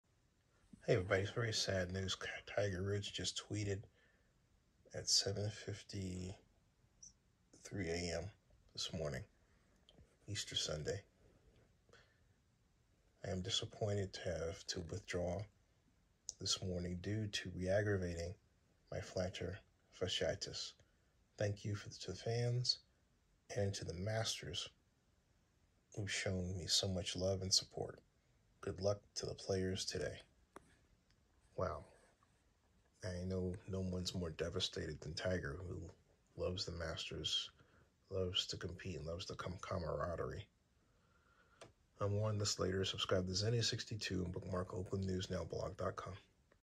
Tiger Woods Withdraws From 2023 Masters After Injury: Reaggravating His Plantar Fasciitis
A very sad Easter Sunday as Tiger Wood announced he withdrew from the 2022 Masters after injury.  Tiger reaggrevated  his planter fasciitis.

On Twitter Tiger thanked the Masters Golf Tournament for its love and support of him.

This is the first time he has ever had to quit The Masters.